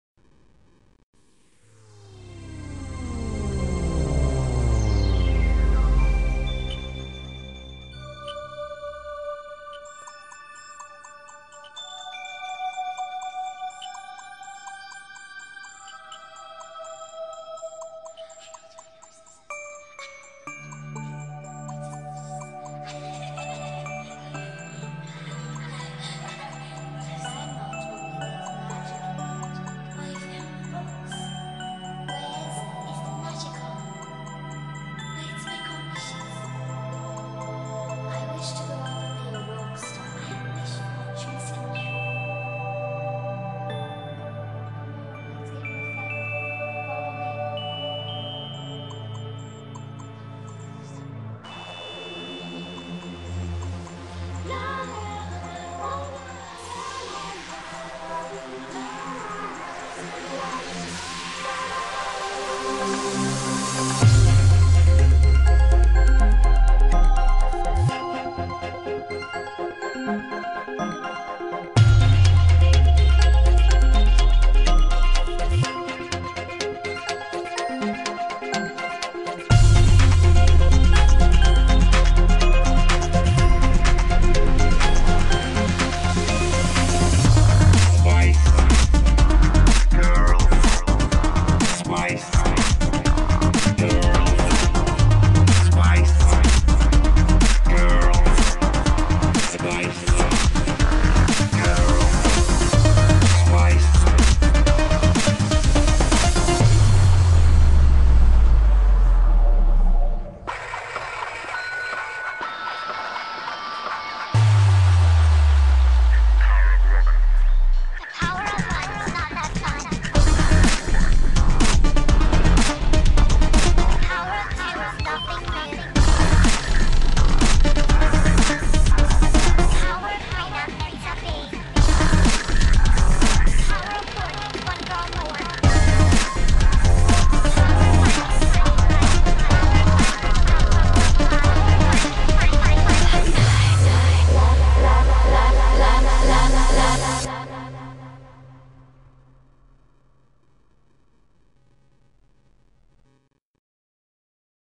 INTRO CONCIERTO SPICE GIRLS FULL VIDEO
EL VIDEO COMPLETO DE LA INTRODUCCION DEL CONCIERTO, EL QUE SE PROYECTA EN LAS PANTALLAS ANTES DE QUE SALGAN LAS CHICAS COMPLETO!